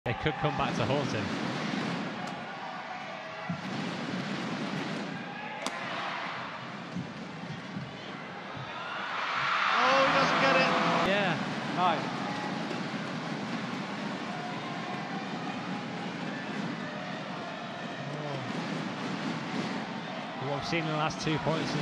Misshed It! Brian Yang vs Anthony ginting
2023年泛美锦标赛冠军杨灿vs2023年亚洲锦标赛冠军金庭
2023 Pan Am Championships Champion vs 2023 Asia Championships Champion
27 January 2024 Indonesia Master 500 Semi Final
Brian Yang vs  Anthony Sinisuka Ginting
加拿大杨灿vs印尼金庭
15:19, Brian Won 6 Points In A Roll, Become the First None Asian or European Players Come To The Final in Supper 500+